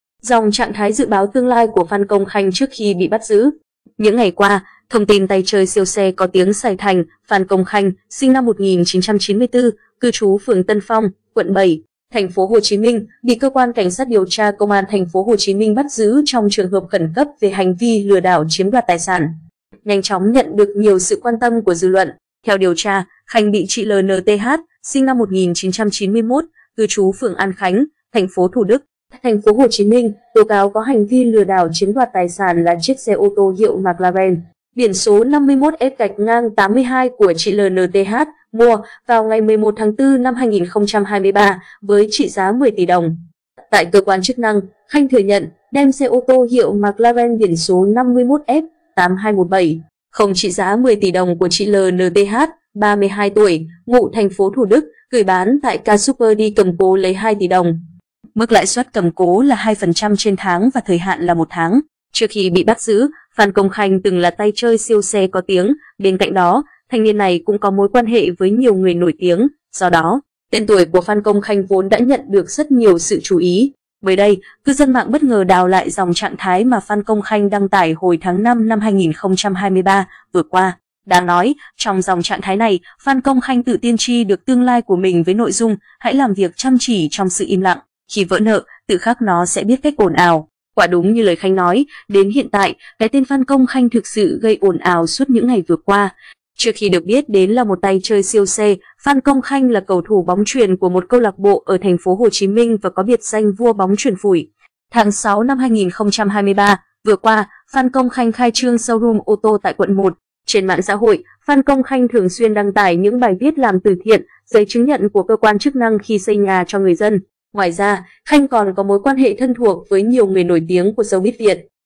Dòng trạng thái dự báo tương lai của Phan Công Khanh trước khi bị bắt giữ
Những ngày qua, thông tin tay chơi siêu xe có tiếng Sài thành - Phan Công Khanh (SN 1994; cư trú phường Tân Phong, quận 7, TP.HCM) bị Cơ quan CSĐT Công an TP.HCM bắt giữ trong trường hợp khẩn cấp về hành vi 'Lừa đảo chiếm đoạt tài sản' nhanh chóng nhận được nhiều sự quan tâm của dư luận.Trước khi được biết đến là một tay chơi siêu xe, Phan Công Khanh là cầu thủ bóng chuyền của một câu lạc bộ ở TP.HCM và có biệt danh vua bóng chuyền phủi.
Quả đúng như lời Khanh nói, đến hiện tại, cái tên Phan Công Khanh thực sự gây ồn ào suốt những ngày vừa qua.
Tháng 6/2023 vừa qua, Phan Công Khanh khai trương showroom ô tô tại quận 1.
Phan Công Khanh
Tại cơ quan chức năng, Khanh thừa nhận đem xe ô tô hiệu McLaren biển số 51F-821.70 trị giá 10 tỷ đồng của chị L.N.T.H. (32 tuổi, ngụ TP.Thủ Đức) gửi bán tại K-Super đi cầm cố lấy 2 tỷ đồng. Mức lãi suất cầm cố là 2%/tháng và thời hạn là một tháng.
Theo điều tra, Khanh bị chị L.N.T.H. (SN 1991; cư trú phường An Khánh, TP Thủ Đức, TP.HCM) tố cáo có hành vi lừa đảo chiếm đoạt tài sản là chiếc xe ô tô hiệu MCLAREN, biển số 51F-82... của chị L.N.T.H. (mua vào ngày 11/4/2023) với trị giá 10 tỷ đồng.
Trên mạng xã hội, Phan Công Khanh thường xuyên đăng tải những bài viết làm từ thiện, giấy chứng nhận của cơ quan chức năng khi xây nhà cho người dân. Ngoài ra, Khanh còn có mối quan hệ thân thuộc với nhiều người nổi tiếng của showbiz Việt.
Trước khi bị bắt giữ, Phan Công Khanh từng là tay chơi siêu xe có tiếng, bên cạnh đó, thanh niên này cũng có mối quan hệ với nhiều người nổi tiếng, do đó, tên tuổi của Phan Công Khanh vốn đã nhận được rất nhiều sự chú ý.
Mới đây, cư dân mạng bất ngờ "đào lại" dòng trạng thái mà Phan Công Khanh đăng tải hồi tháng 5/2023 vừa qua. Đáng nói, trong dòng trạng thái này, Phan Công Khanh tự "tiên tri" được tương lai của mình với nội dung: "Hãy làm việc chăm chỉ trong sự im lặng. Khi vỡ nợ, tự khắc nó sẽ biết cách ồn ào".